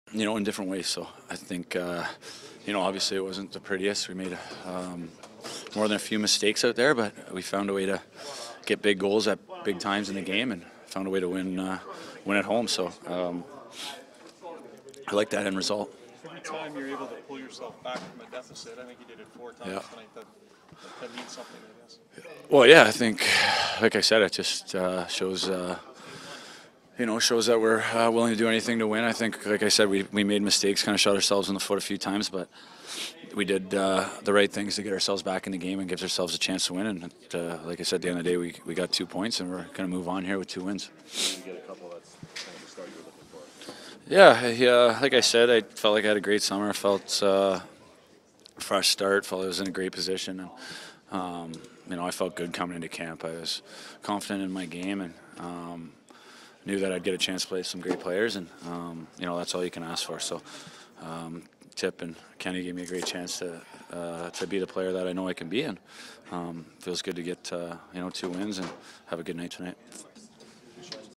POST-GAME RAW | James Neal 10.05.19
James Neal speaks to the media following the Oilers 6-5 win over the Kings